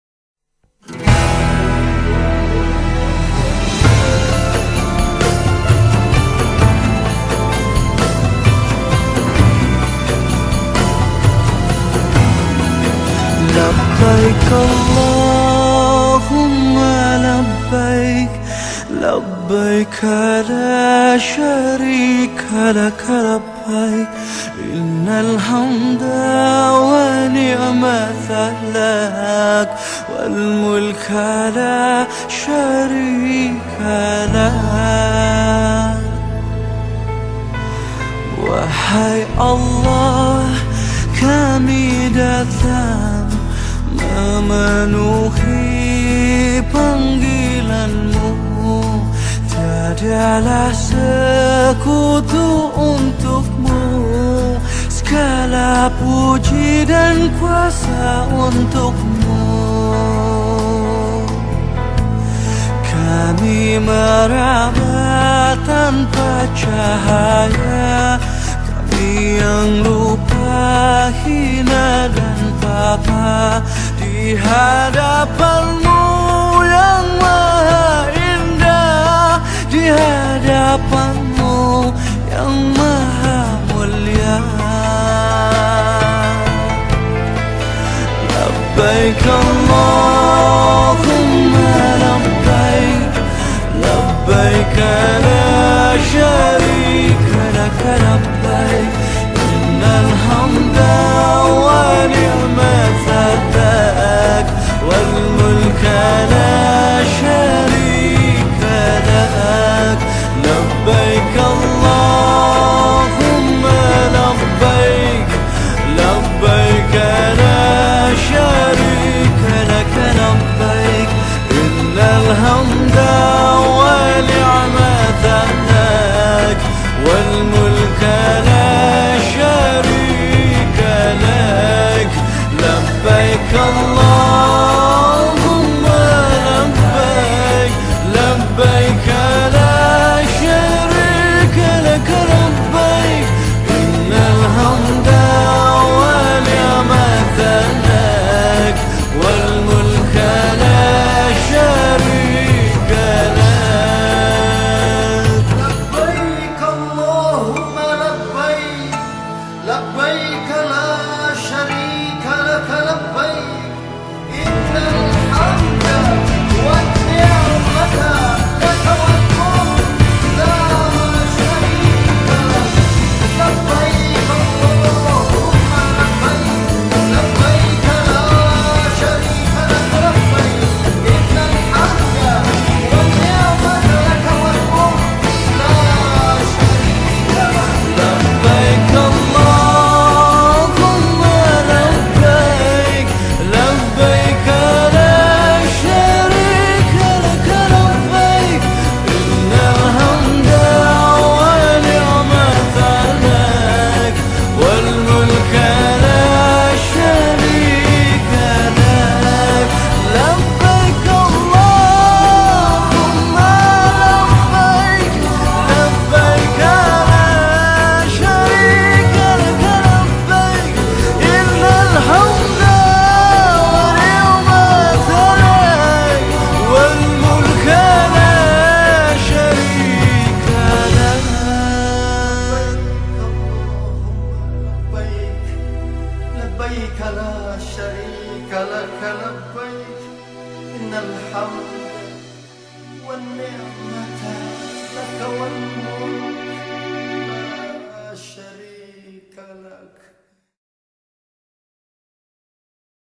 Umroh di Bulan Suci Ramadan
Pengalaman Uroh dissat Bulan Suci Ramadan. Thanks GOD My dreams was come true, I'm so lucky visited YOUR HOME. I can't say anything when I was there only thinking, that was I dreaming or not ? but that was real :)